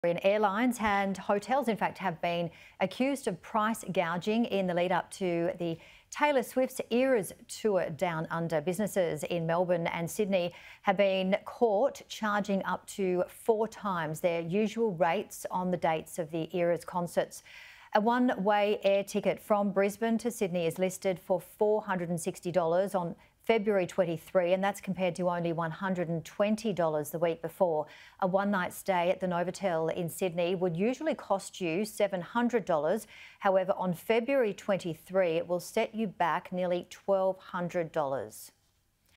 Airlines and hotels accused of price gouging for Taylor Swift's Eras Tour in Australia
Airlines and hotels have been accused of price gouging in the lead-up to Taylor Swift’s Eras Tour Down Under.

Businesses in Melbourne and Sydney have been caught charging up to four times their usual rates on the dates of the Eras concerts.

A one-way air ticket from Brisbane to Sydney is listed for $460 on February 23, compared to only roughly $120 the week before.

A one-night stay at the Novotel in Sydney would usually cost you $700, but on February 23, it will set you back nearly $1200.

The pop star is set to begin her Eras Tour Down Under on Friday, the 16th of February, and will travel to Melbourne first before heading to Sydney for her show.